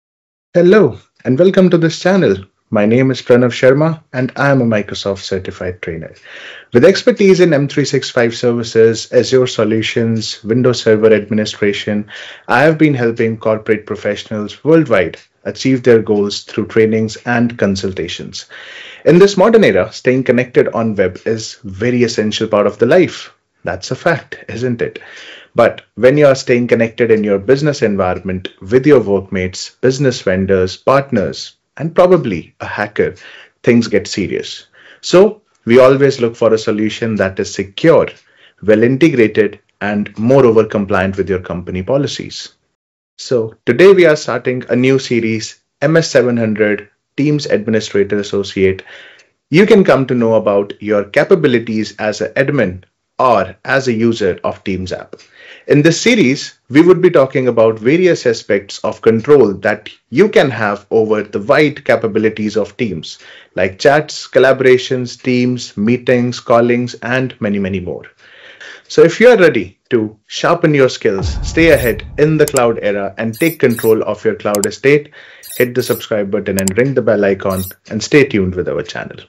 MS-700 Certification Introduction | Microsoft Teams Admin Exam Overview & Speaker Intro
Kickstart your journey to becoming a certified Microsoft Teams Administrator with this MS-700 exam introduction!
In this video, meet our expert speaker and get a detailed overview of the MS-700 certification – your gateway to mastering Microsoft Teams deployment, management, and security. Whether you’re an IT professional, Teams admin, or aspiring cloud specialist, this session will help you understand the exam structure, skills measured, and career benefits of MS-700.

✅ What you’ll learn:
Speaker introduction & expertise
MS-700 certification overview
Key skills measured in the exam
Microsoft Teams management fundamentals
Career growth opportunities in Microsoft 365